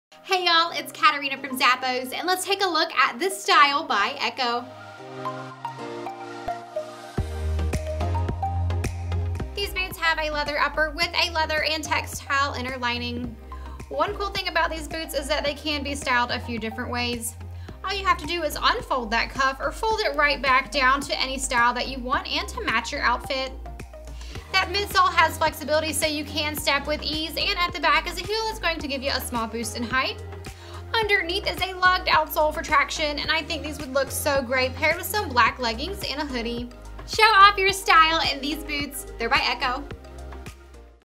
ECCO Tredtray Pull-On Boot SKU: 9423105
ECCO Womens Size Chart
A moto-inspired look, The ECCO&reg; Tredtray Pull-On Boot is the ultimate choice for wintery days. Built for cold weather, the design features a fold-down option, exposing the natural shearling for a different look. Designed with style in mind, this will become your go-to cold-weather choice when trying to maintain function and fashionable appeal.
ECCO FLUIDFORM&trade; is a direct injection process that uses fluid materials to form a lightweight, flexible, and cushioned sole.
Constructed with a premium leather upper, a shearling wool lining and cuff, and a durable synthetic outsole with a lug tread.
This product contains real fur from Sheep or Lamb.Fur Origin: Slovakia.Real Fur has been artificially dyed and treated.

Product measurements were taken using size 39 (US Women's 8-8.5), width M. Please note that measurements may vary by size.